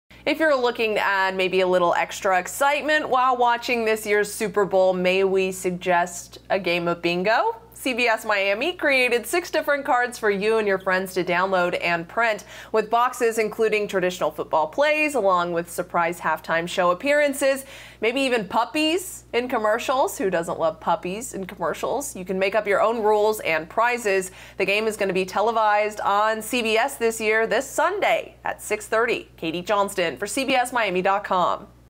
Big Game Bingo: Play Along As You Watch Super Bowl 55 In Tampa
Super Bowl Sunday may look and feel a little different this year due to the COVID-19 pandemic, but if you are having a few friends over (be sure to wear a mask), or if you are enjoying the game with family and friends virtually, be sure to print these special Big Game Bingo cards to play at home as the action unfolds on the field! Katie Johnston reports.